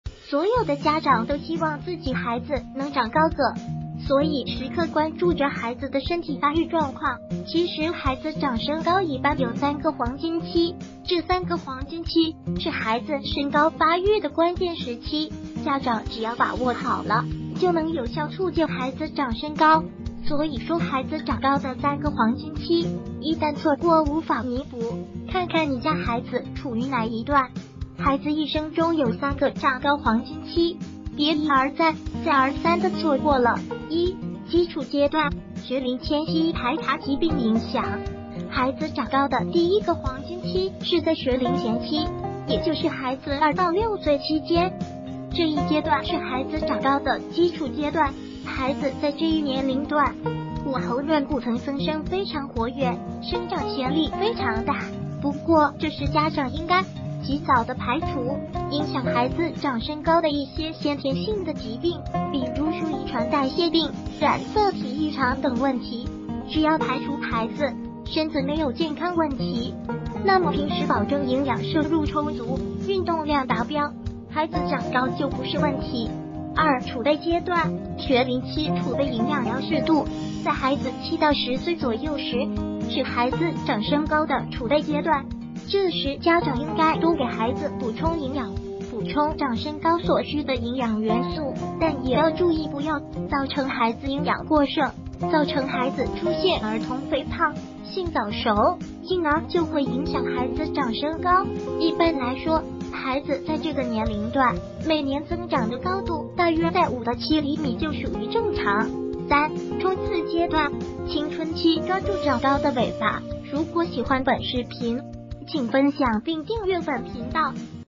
孩子一生中有3個“長高黃金期”，別一而再、再而三的錯過了！
孩子一生中有3個“長高黃金期”，別一而再、再而三的錯過了！長身高 長高 青春期 黃金期孩子一生中有3個“長高黃金期”，別一而再、再而三的錯過了！育兒錦囊 育兒